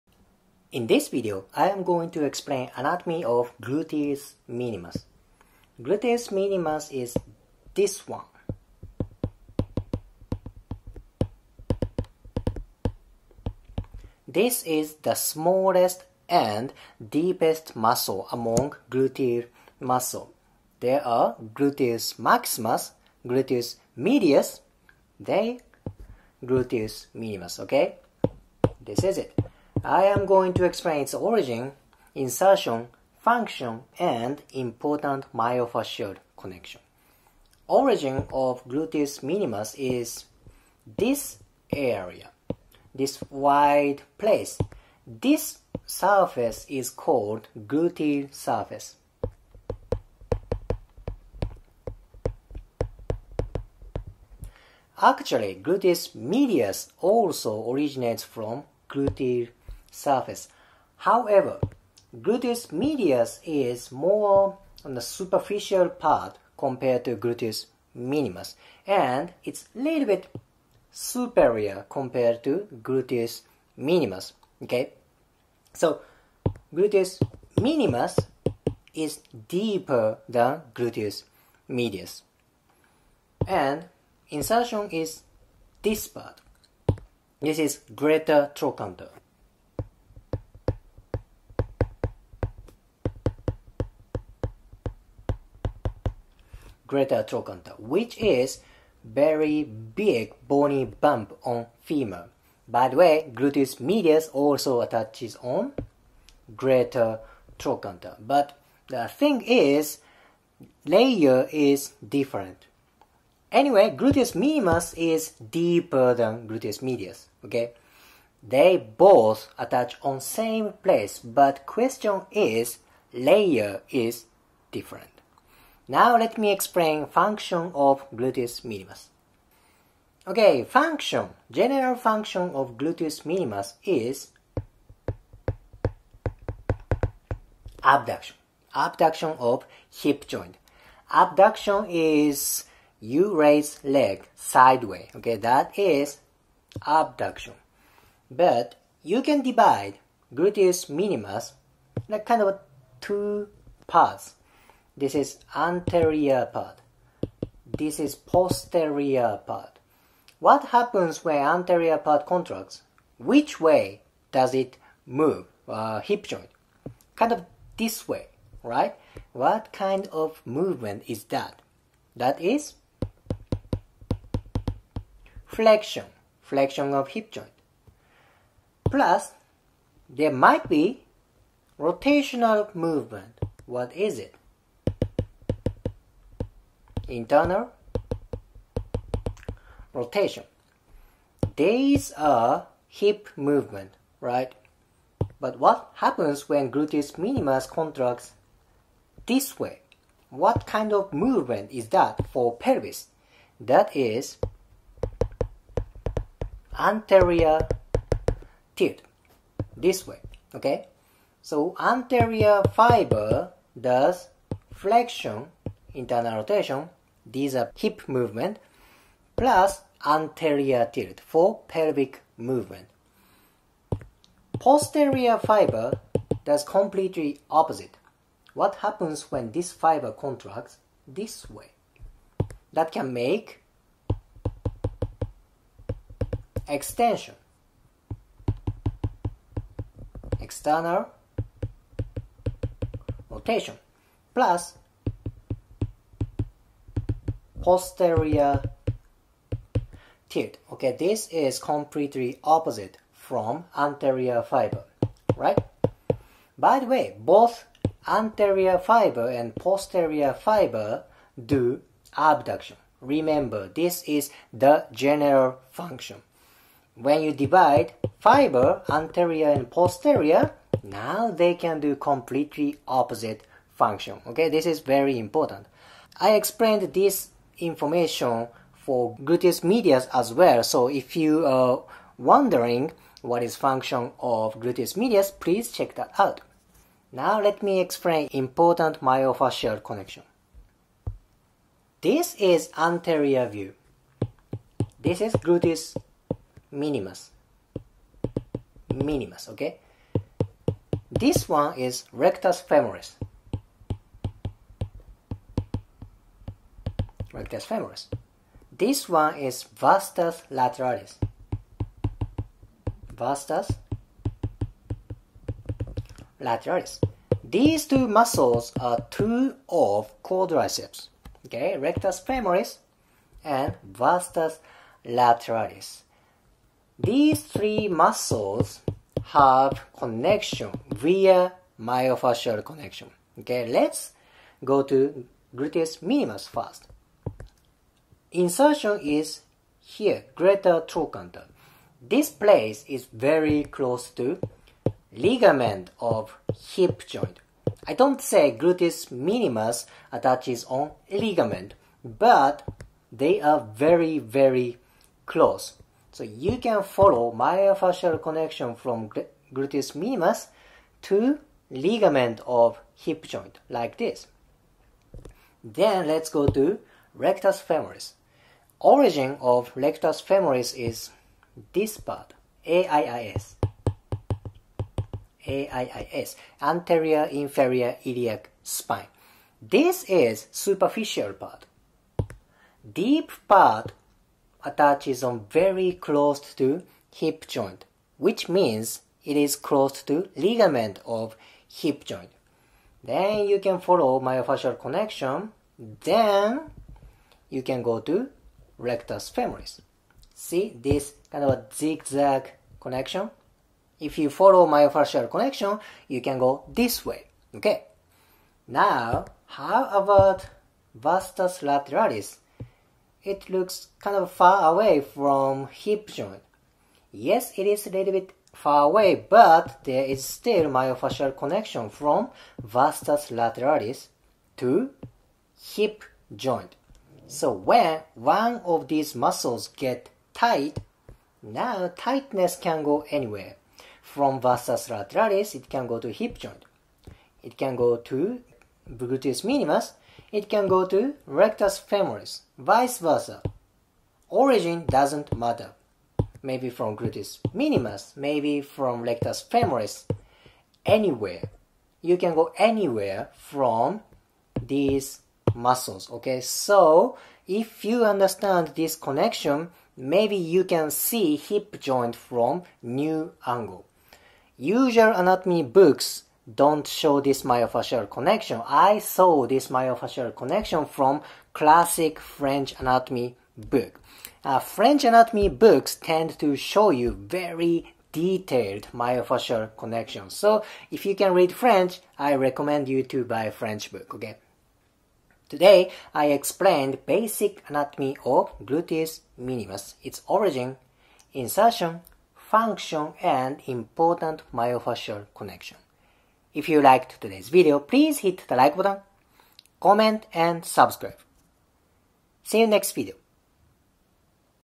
Anatomy and function of gluteus minimus
In this video, I explain anatomy of gluteus minimus..